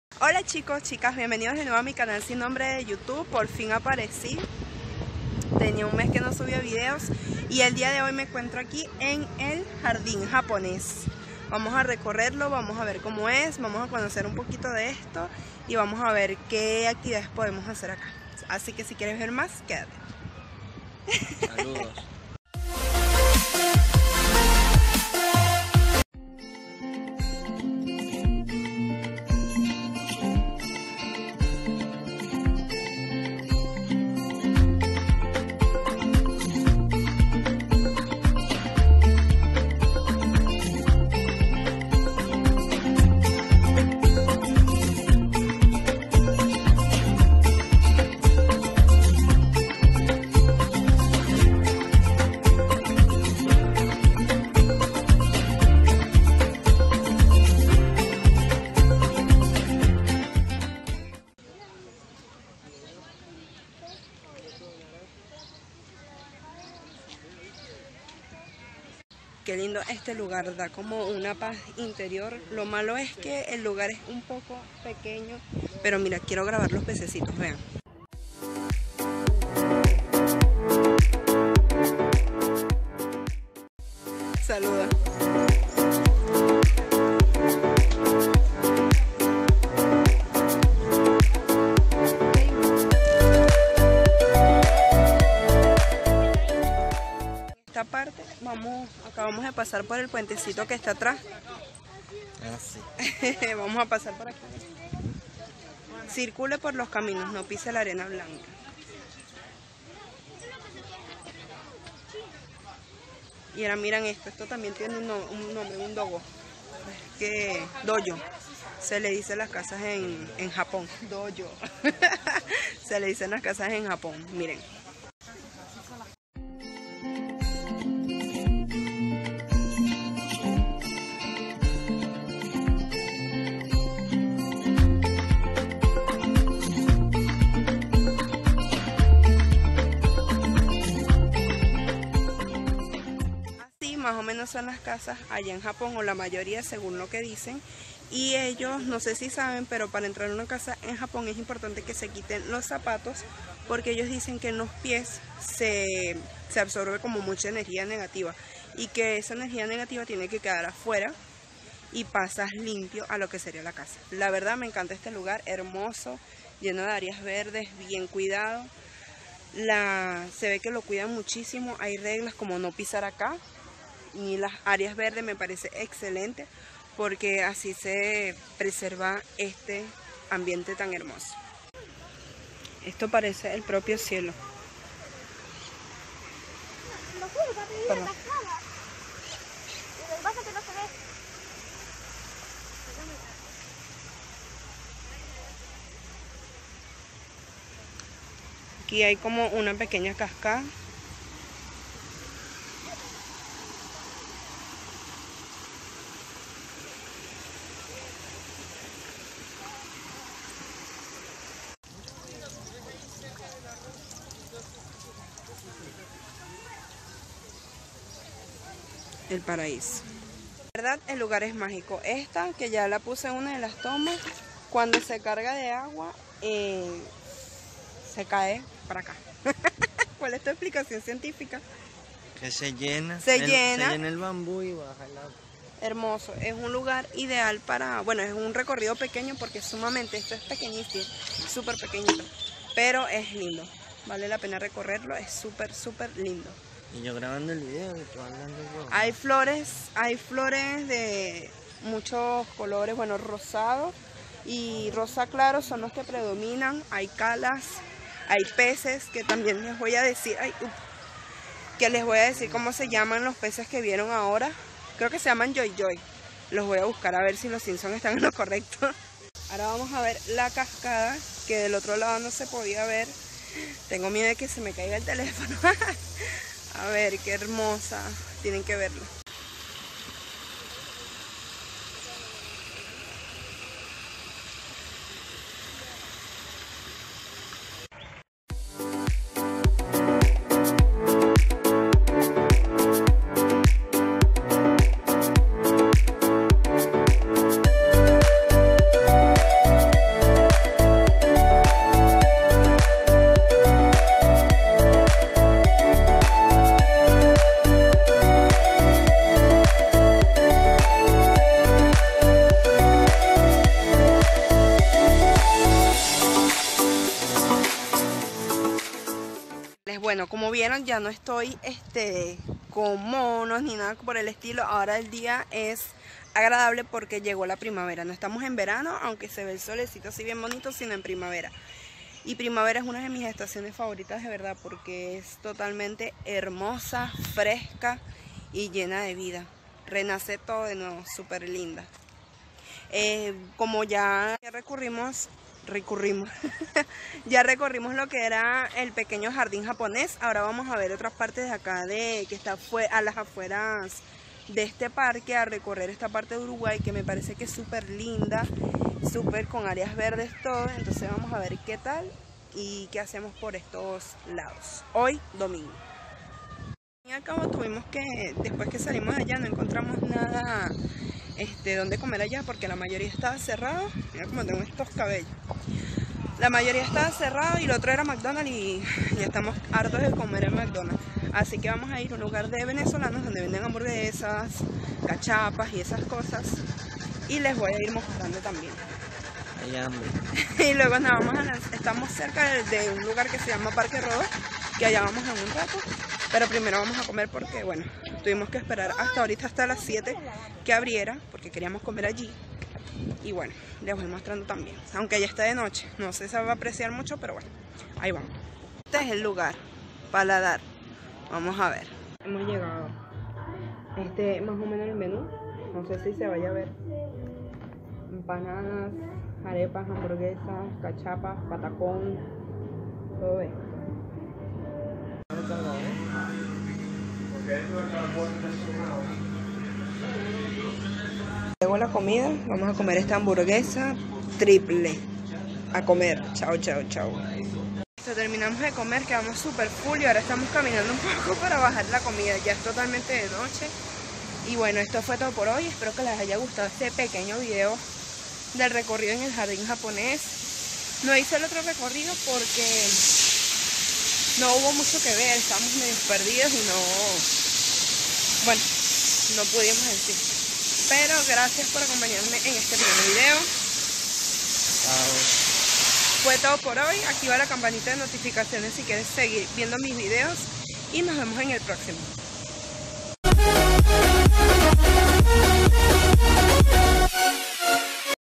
¡Ha llegado la primavera! y me fui al jardín japonés/ Uruguay
Estuve de visita en El jardín Japonés en Uruguay. ¡Eres importante en este proceso! 😚😙😗😍😍